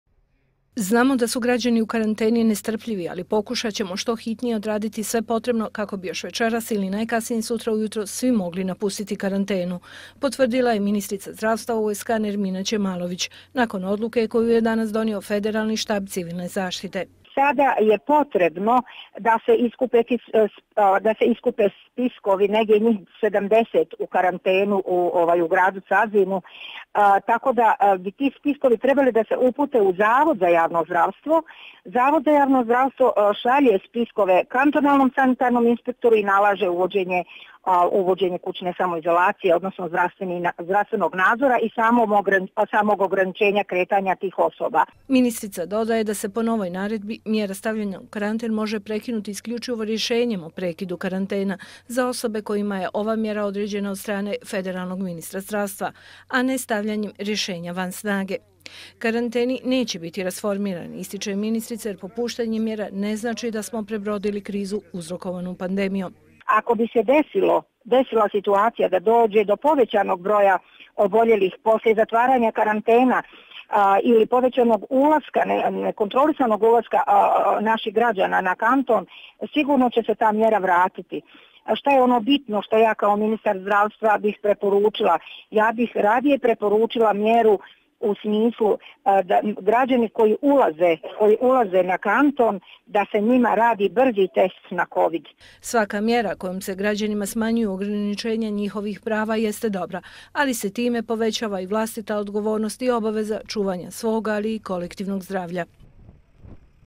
MEDRESA U CAZINU: Nove odredbe donijele malo optimizma privremeno smještenim u karantenu
Izvor: RTV USK